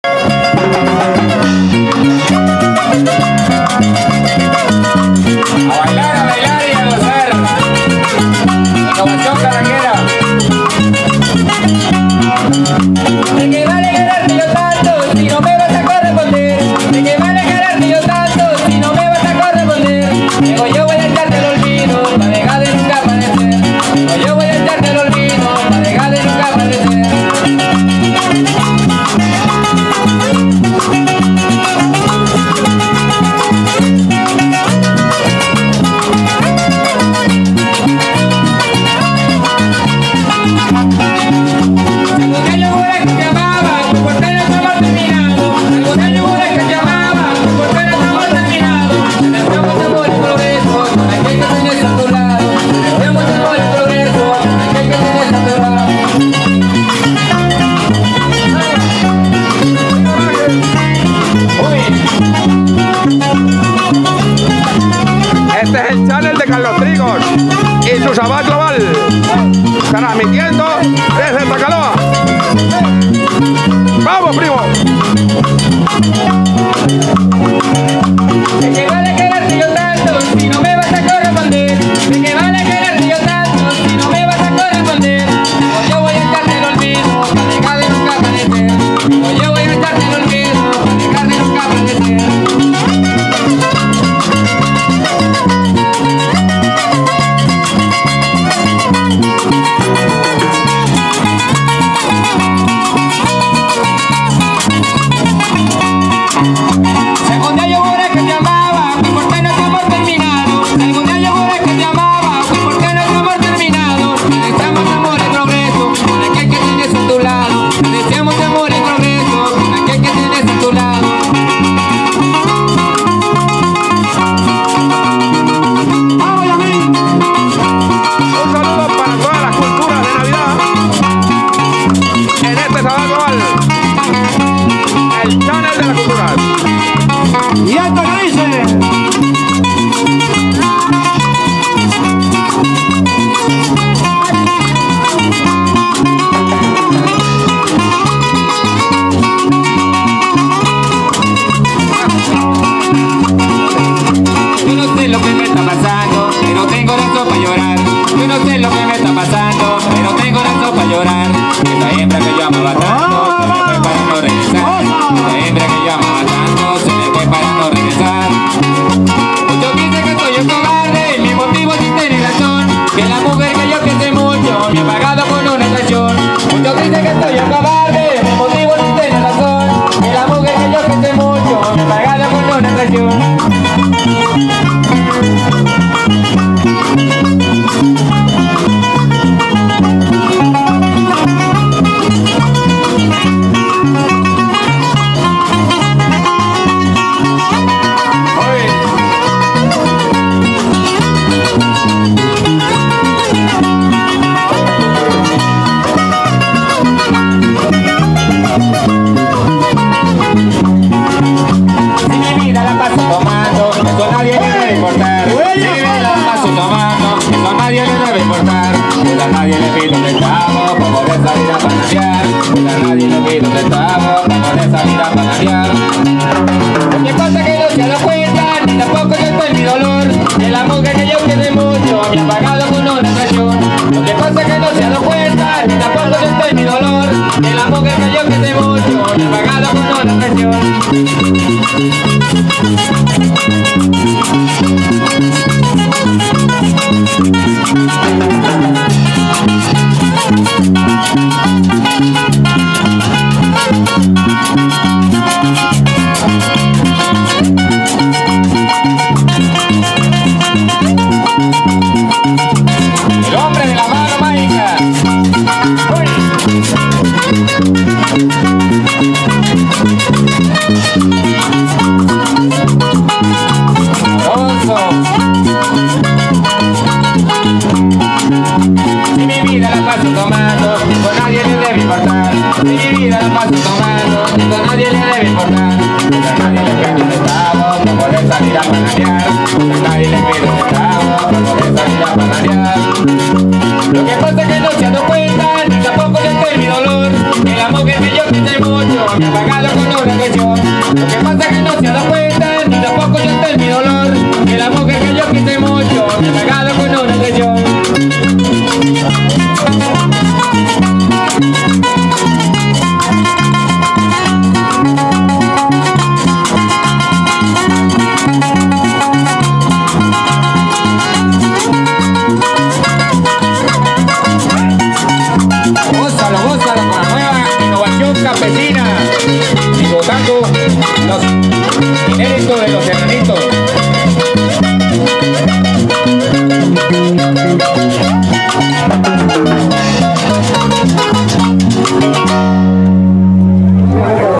ARTISTA INVITADO - C C J G
ORANGEL RINCÓN, nos está deleitando en esta tarde de sábado, en Tacaloa, donde se ha convertido en una plaza de desfile de poetas, compositores y artistas.  Nuestro CHANNEL SEÑAL CATATUMBO GLOBAL, desde este santuario.